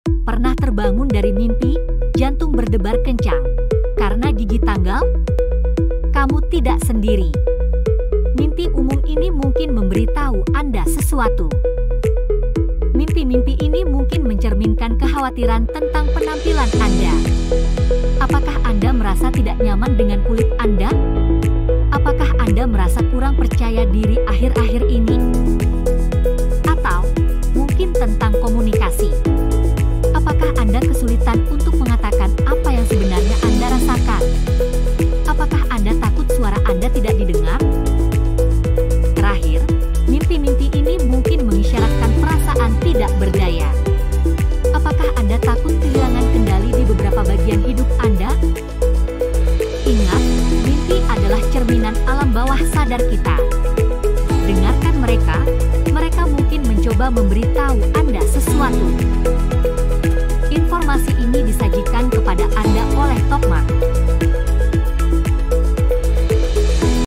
Terjemahkan Mimpi Anda: Makna Gigi Gugur yang Dijelaskan! 😱
Pernahkah Anda bermimpi gigi Anda rontok? 🦷😬

Pernah bertanya-tanya apa artinya?

 Dalam YouTube Shorts singkat ini, kami akan menjelajahi dunia yang menarik dari interpretasi mimpi dan mengungkap simbolisme tersembunyi di balik gigi yang rontok tersebut.

Jangan lewatkan untuk memahami rahasia yang mungkin terungkap oleh mimpi Anda! 😴💭